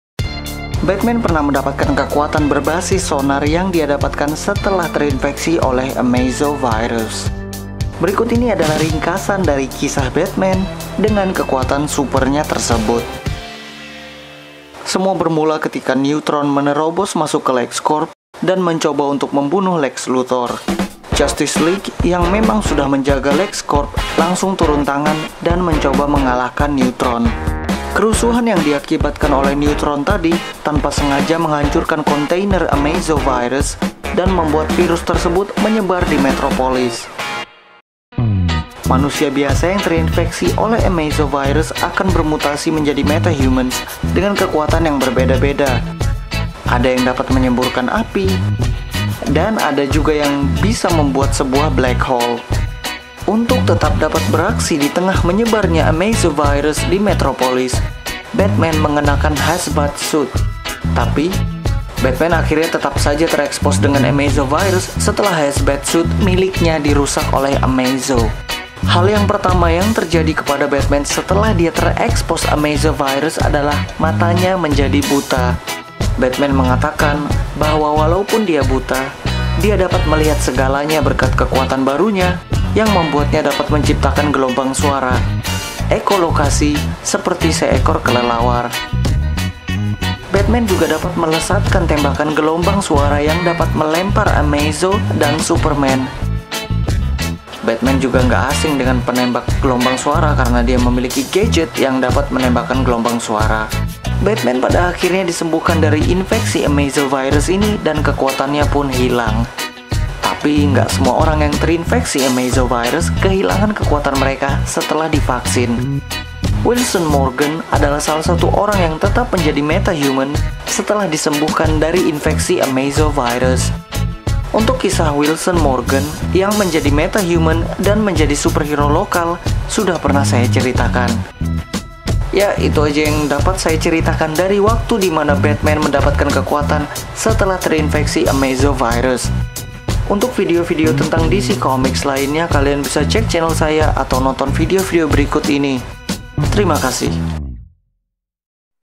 Batman Dengan Kekuatan Manipulasi Sonar
Batman pernah mendapatkan kekuatan berbasis sonar yang dia dapatkan setelah terinfeksi oleh Amazo virus. Berikut ini adalah ringkasan dari kisah Batman, dengan kekuatan supernya tersebut.